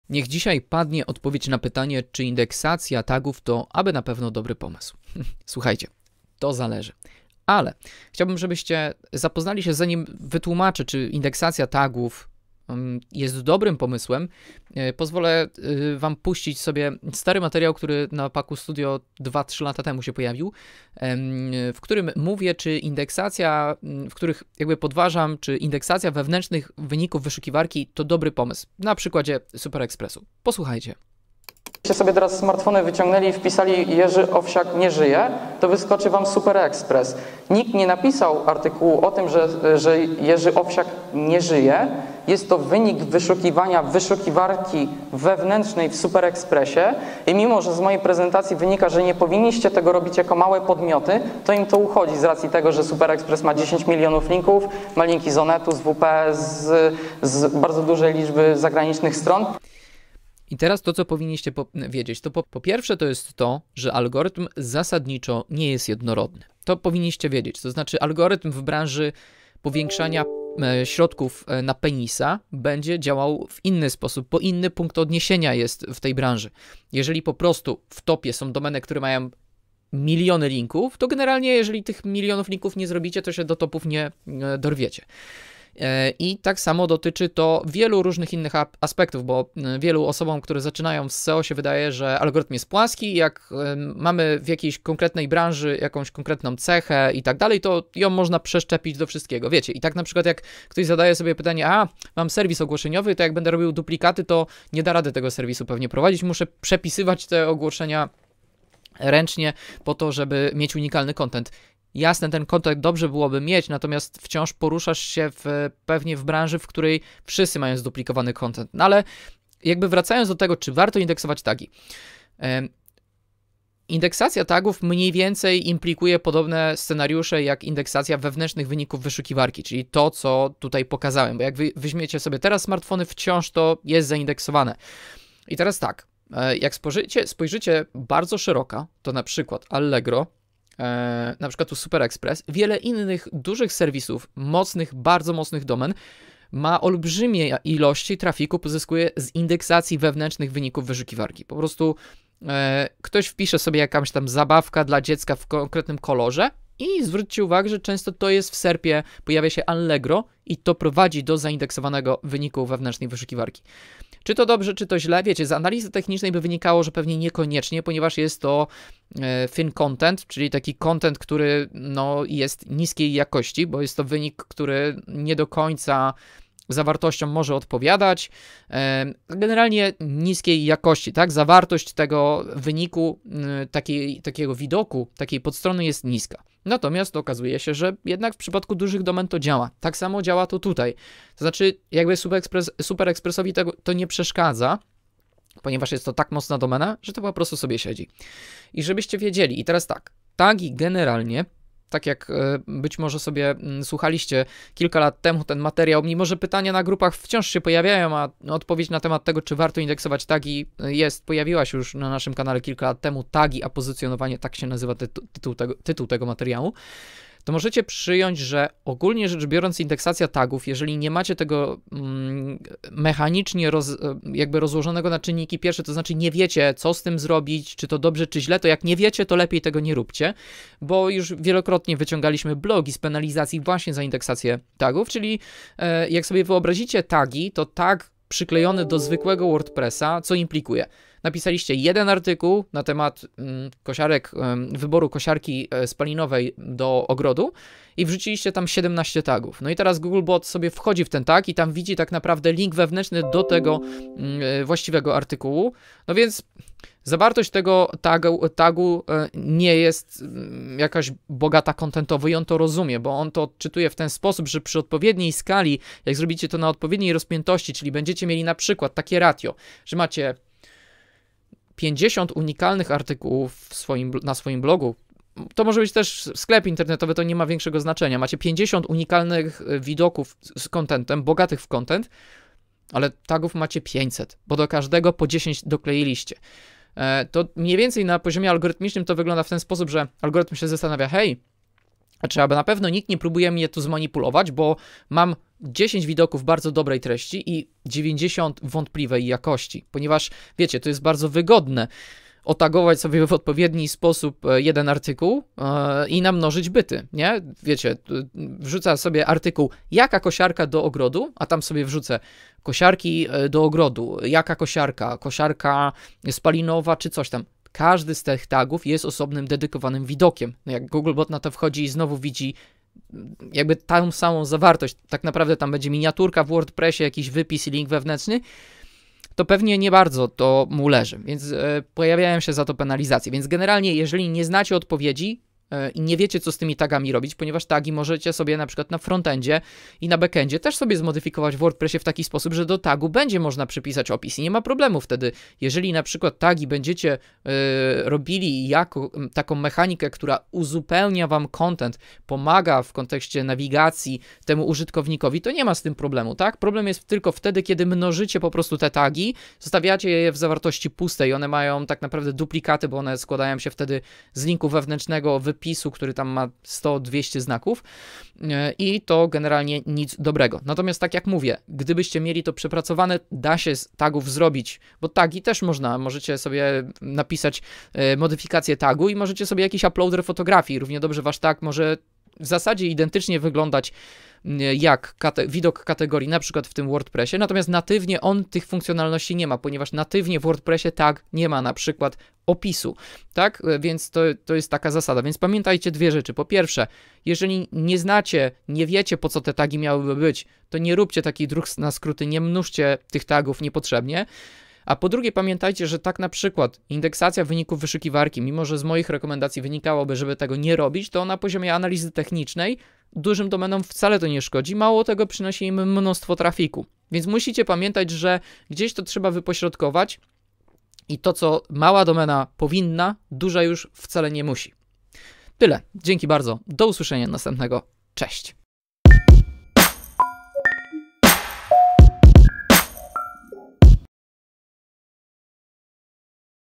Czy warto indeksować tagi i wyniki wyszukiwania?
Czy indeksacja tagów to, aby na pewno dobry pomysł? Co z wynikami wyszukiwania? 🎯 Co wolno małym wydawcom, a co właścicielom silnych domen o ugruntowanej pozycji rynkowej? W tym wideo Adrian Pakulski tłumaczy niuanse, w jakich okolicznościach nadużywanie tych mechanik może prowadzić do penalizacji i wytracenia w zasadzie 100% zasięgów organicznych w Google 🤯